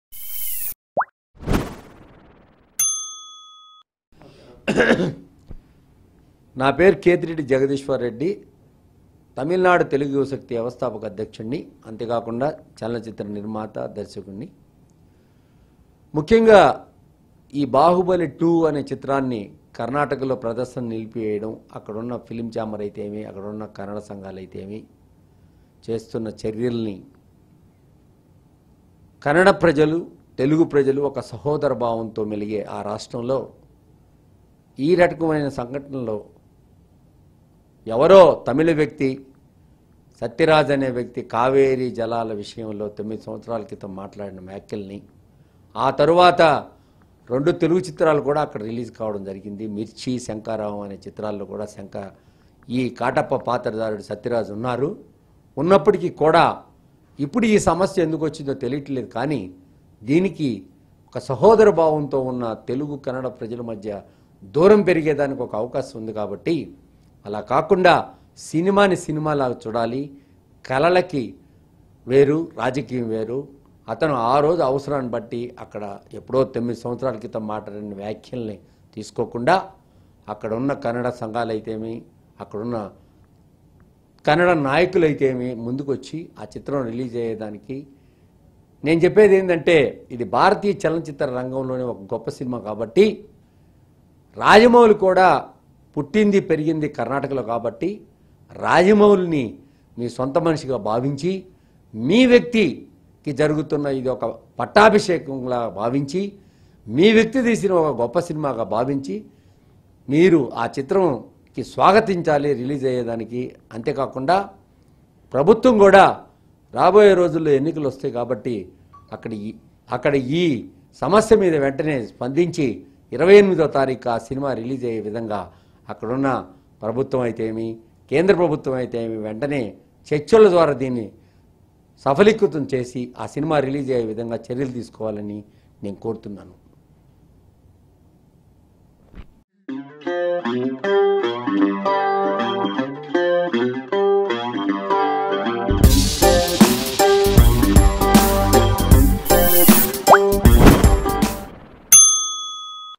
Kethireddy Jagadeeswar Reddy About Baahubali 2 Ban in Karnataka | Silly Monks
Kethireddy Jagadeeswar Reddy About Baahubali 2 Ban in Karnataka